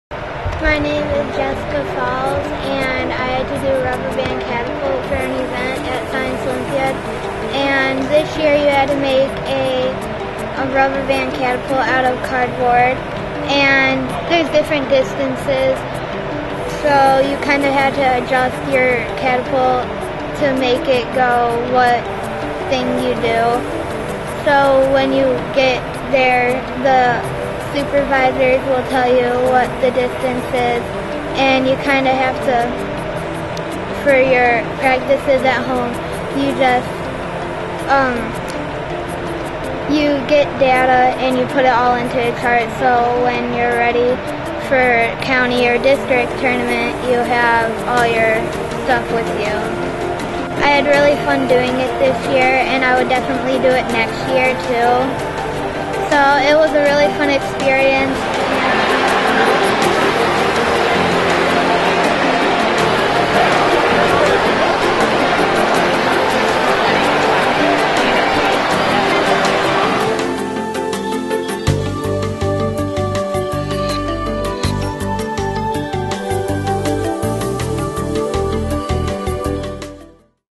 Science Olympiad Rubber Band Catapult
A glimpse of Rubber Band Catapult, one of 16 events at the Macomb Science Olympiad tournament. 80 elementary schools (both public and private) compete.

Macomb Science Olympiad is a 501c3 run entirely by volunteers.